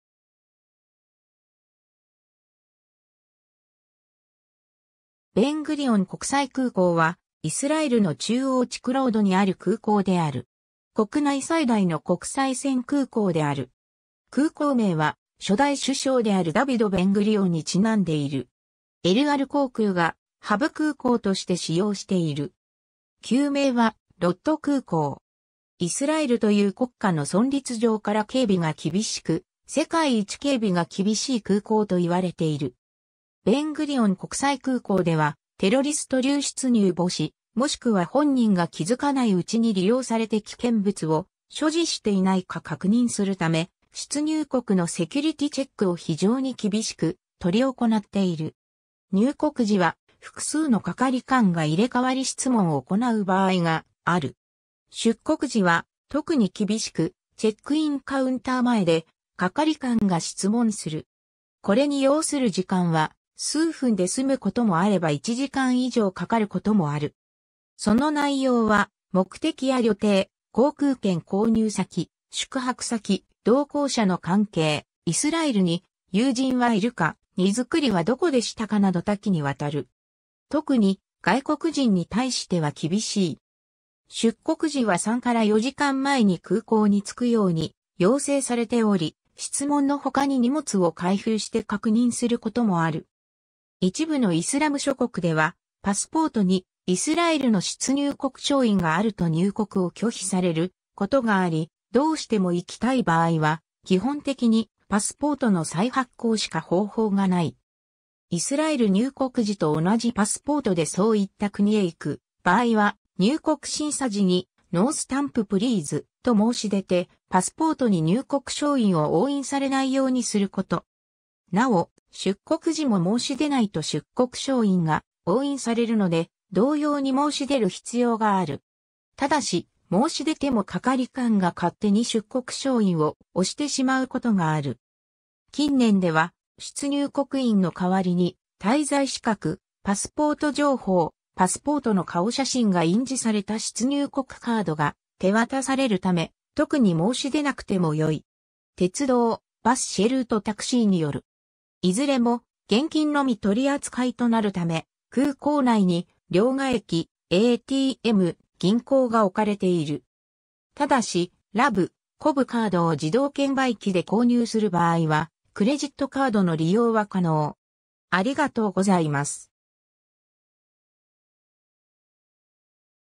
ベン・グリオン国際空港
中央地区_(イスラエル)
ダヴィド・ベン＝グリオン
ベン・グリオン国際空港（ベン・グリオンこくさいくうこう、ヘブライ語：נמל תעופה בן גוריון‎、英語: Ben Gurion International Airport）は、イスラエルの中央地区ロード（ロッド）にある空港である。
 国内最大の国際線空港である。
空港名は初代首相であるダヴィド・ベン＝グリオンにちなんでいる。
エル・アル航空がハブ空港として使用している。
旧名はロッド空港。
 イスラエルという国家の存立上から警備が厳しく、世界一警備が厳しい空港と言われている。
 ベン・グリオン国際空港ではテロリスト流出入防止、もしくは本人が気づかないうちに利用されて危険物を所持していないか確認するため、出入国のセキュリティチェックを非常に厳しく執り行っている。
入国時は複数の係官が入れ替わり質問を行う場合がある。
出国時は特に厳しく、チェックインカウンター前で係官が質問する。
これに要する時間は数分で済むこともあれば1時間以上かかることもある。
その内容は目的や旅程、航空券購入先、宿泊先、同行者の関係、イスラエルに友人はいるか、荷造りはどこでしたかなど多岐に渡る。
特に、外国人（非イスラエル人）に対しては厳しい。
 出国時は3～4時間前に空港に着くように要請されており、質問の他に荷物を開封して確認することもある。
 一部のイスラム諸国（特にイスラエルと直接戦争している国）では、パスポートにイスラエルの出入国証印があると入国を拒否されることがあり、どうしても行きたい場合は基本的にパスポートの再発行しか方法がない。
イスラエル入国時と同じパスポートでそういった国へ行く場合は、入国審査時に「No Stamp Please.」と申し出てパスポートに入国証印を押印されないようにすること。
尚、出国時も申し出ないと出国証印が押印されるので、同様に申し出る必要がある。
ただし、申し出ても係官が勝手に出国証印を押してしまうことがある。
近年では、出入国印の代わりに、滞在資格（期限）、パスポート情報、パスポートの顔写真が印字された出入国カードが手渡されるため、特に申し出なくても良い。
 鉄道・バス・シェルート（乗合タクシー）・タクシーによる。
（2019年現在）いずれも現金のみ取り扱いとなるため、空港内に両替機・ATM・銀行が置かれている。
ただし、Rav-Kavカードを自動券売機で購入する場合は、クレジットカードの利用は可能。